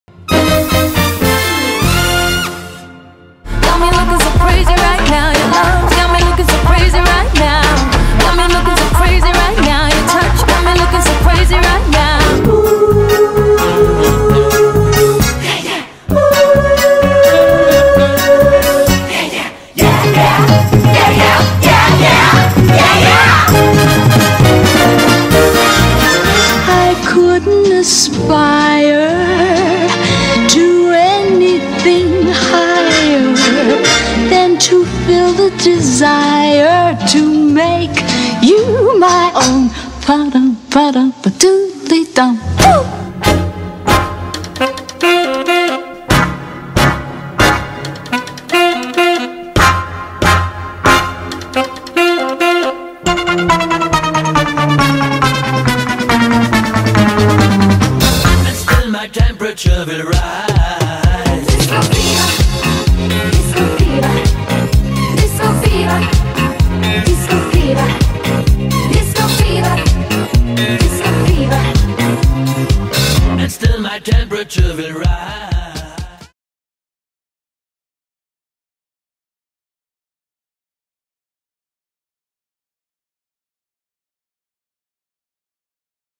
Шоу балеты Москвы
GOLD DIGGERS Официальный сайт.

Сегодня забыть обо всех неприятностях и с головой окунуться в атмосферу американских 20-х можно только в компании шоу-балета GOLD DIGGERS («Золотоискательницы»), который стал неотъемлемой частью лучших московских вечеринок в стиле «Великого Гэтсби» и «Чикаго». Потому что GOLD DIGGERS – это то золото, которого не найдешь на Аляске, это самые красивые девушки в самых блестящих платьях и с самыми изящными прическами. И танцуют эти девушки так, что сам Великий Гэтсби сделал бы их своим домашним шоу-балетом!